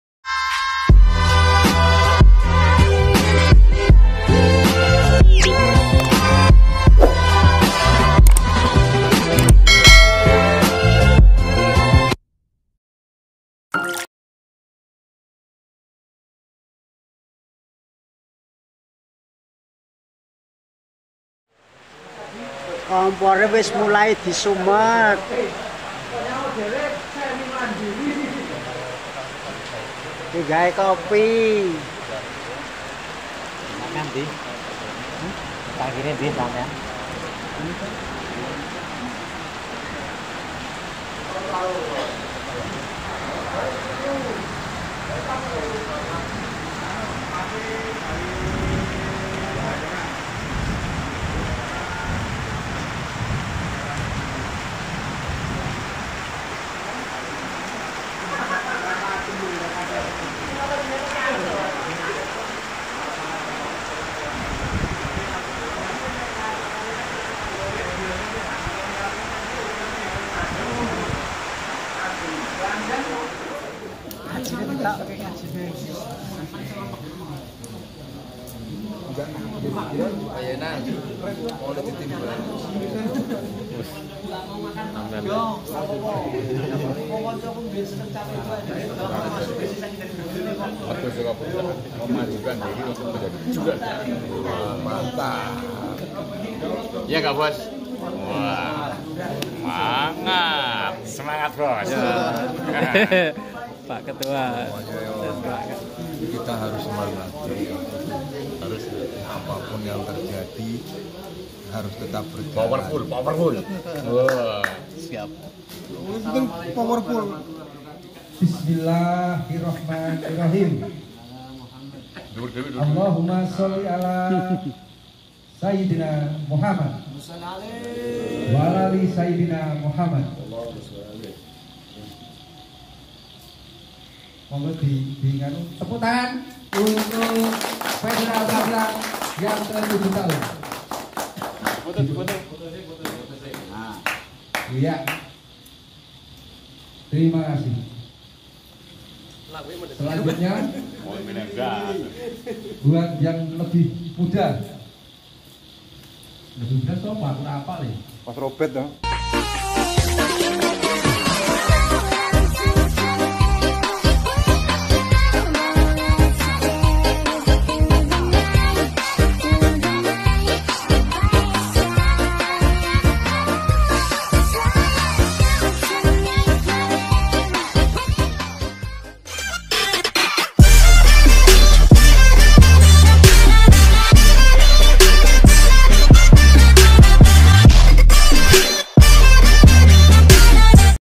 Anniversary Federal Magelang yang ke 7||Tumpengan
terimakasih atas kehadiran dulur ² federalis
temanggung, Salatiga,Jogja dan katula

anniversary federal magelang yg ke 7 Alhamdulillah diberikan kelancaran dan sukses berkah hujan

dan kita lanjut dipagi hari untuk menghadiri acara opening Wedi ngelih tonton terus ya video selanjutnya

dan jangan lupa bantu support channel saya
-komen